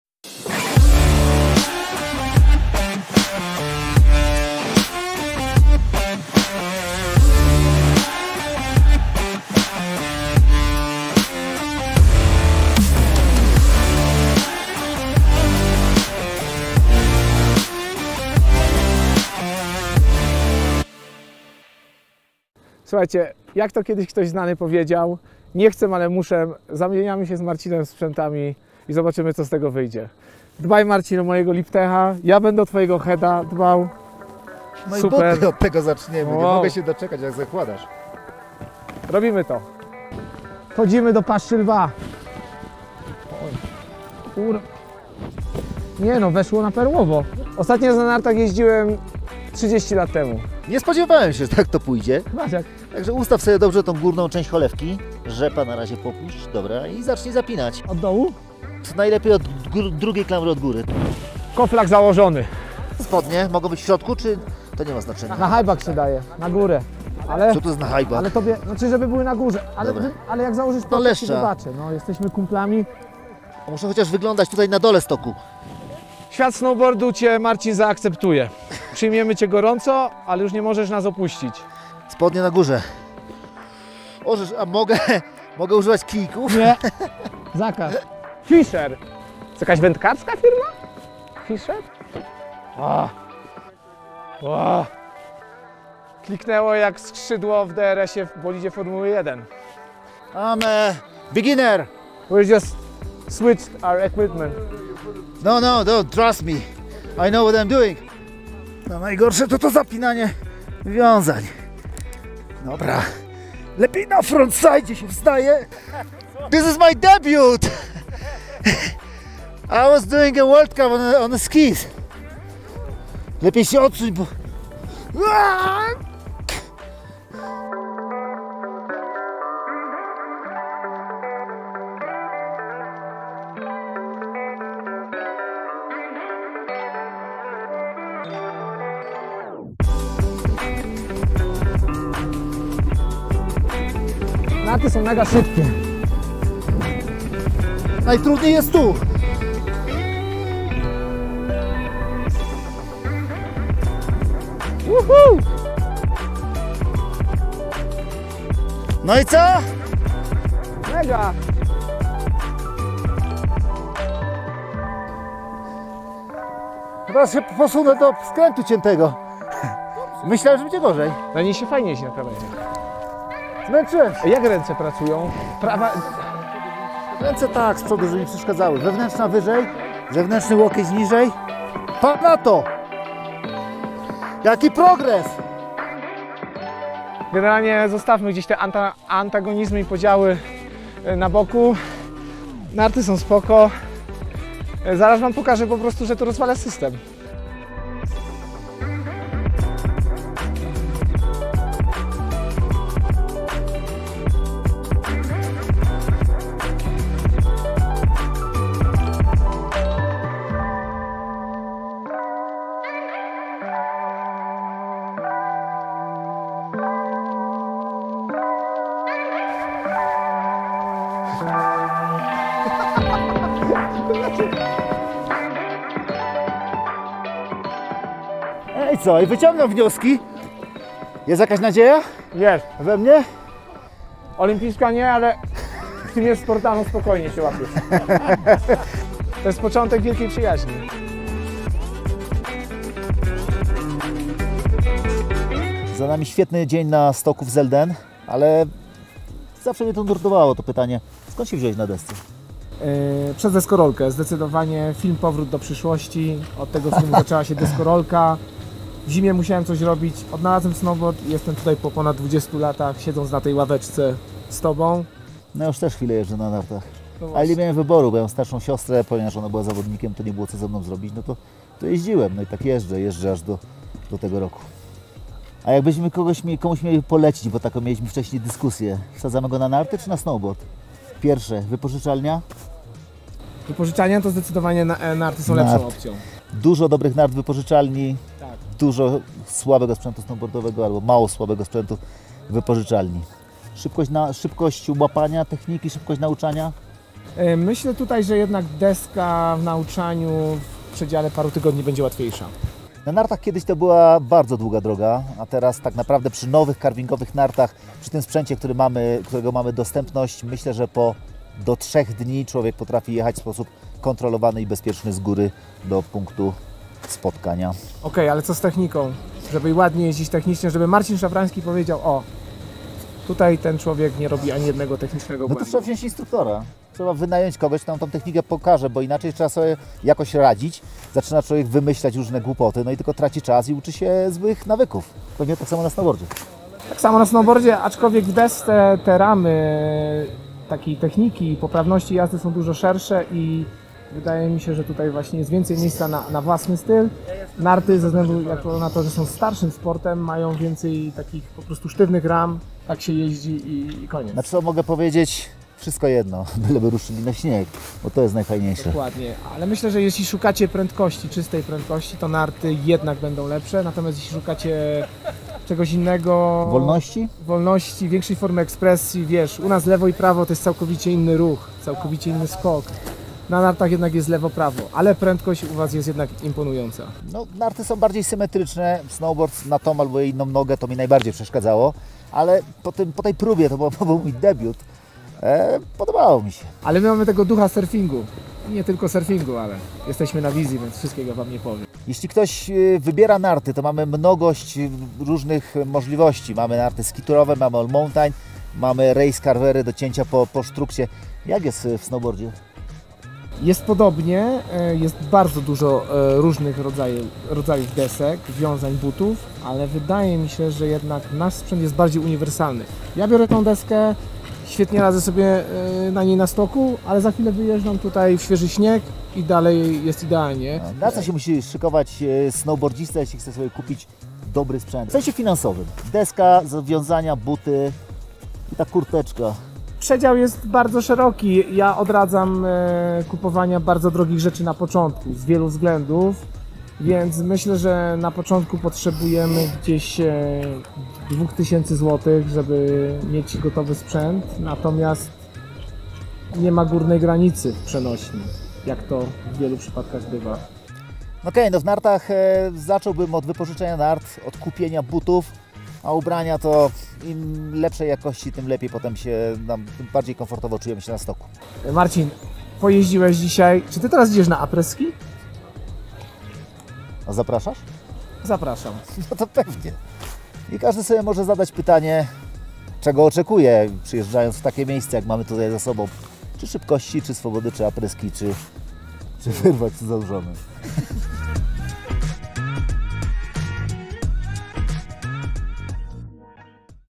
Deska czy narty - od czego zacząć?
Znacie jakieś żarty o snowboardzistach i narciarzach?
Czy ta odwieczna wojna między "ołówkami" i "deskarzami" jest nadal aktualna?
Od czego w ogóle zacząć swoją przygodę ze sportami zimowymi?
Na te pytania odpowiedzą nasi olimpijczycy: Marek Sąsiadek i Marcin Szafrański, dlatego warto zostać do końca!

A po obejrzeniu, poza dowcipami, koniecznie podzielcie się swoją historią z początków przygody ze sportami zimowymi!